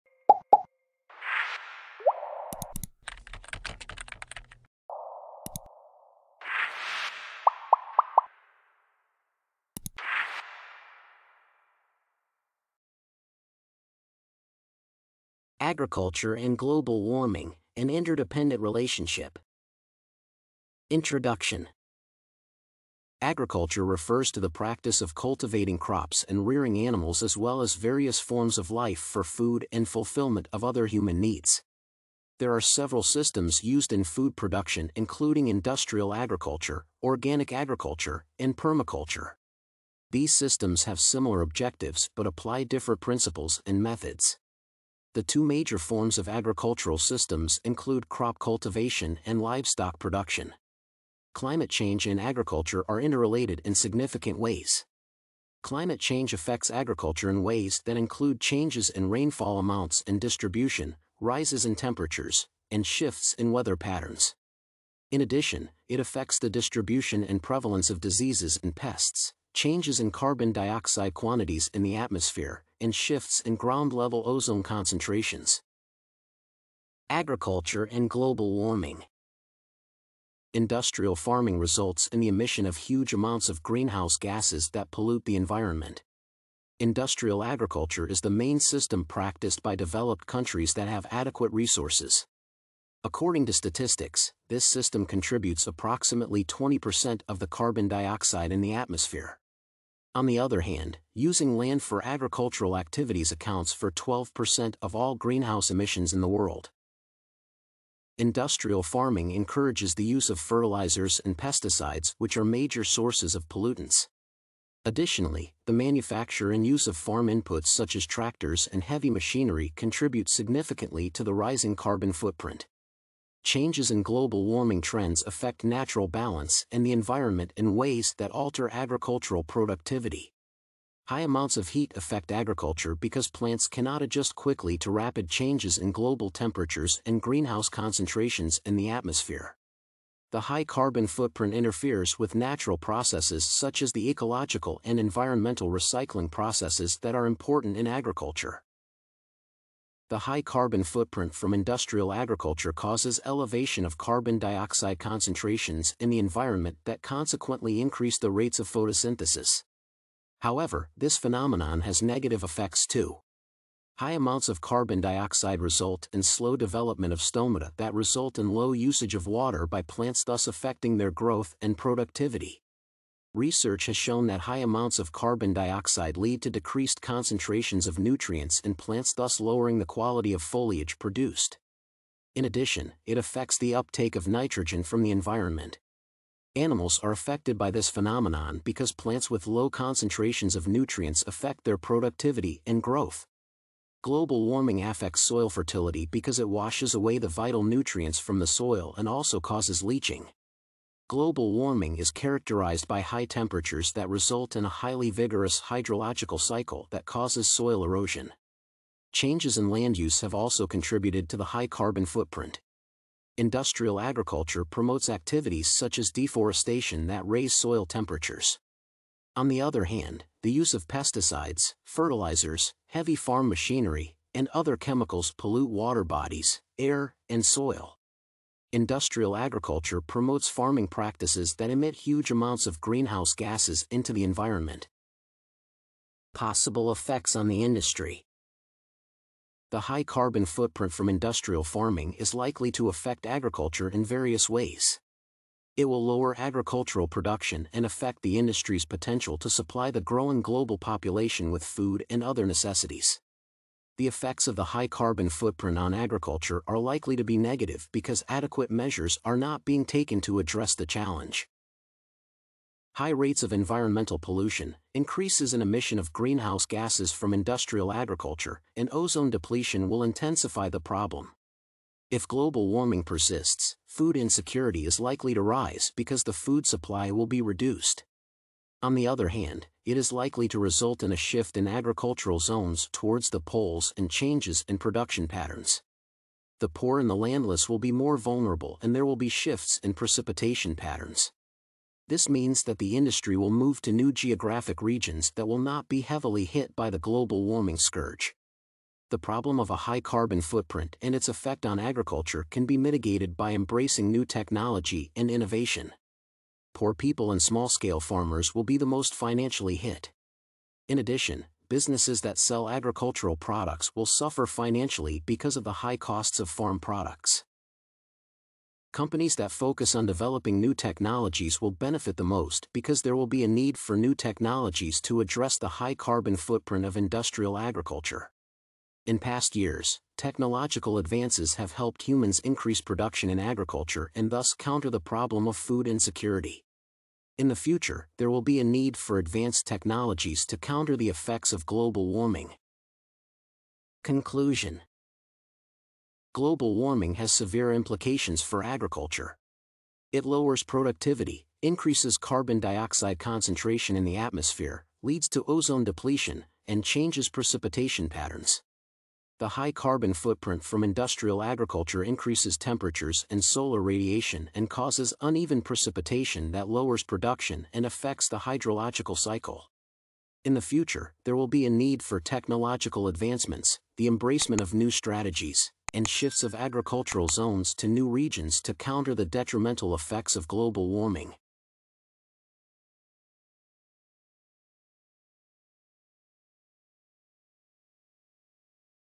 Agriculture and Global Warming: An Interdependent Relationship - Essay Example
📝 Essay description: Climate change and agriculture are interrelated in significant ways. Climate change affects agriculture in ways that include changes in rainfall amounts and distribution, rises in temperatures, and shifts in weather patterns.

ℹ️ Subjects: Sciences, Tech & Engineering
ℹ️ Words: 894
ℹ️ Pages: 4

📚 Reference list:
Johansen, Bruce. The Global Warming Desk Reference. New York: Greenwood Publishing Group, 2002. Print.
Lobell, David, and Burke Marshall. Climate Change and Food Security: Adapting Agriculture to a Warmer World. New York: Springer Science & Business Media, 2009. Print.
Mendelsohn, Robert, and Dinar Ariel. Climate Change and Agriculture: An Economic Analysis of Global Impacts, Adaptation and Distribution Effects. New York: Edward Elgar Publishing, 2009. Print.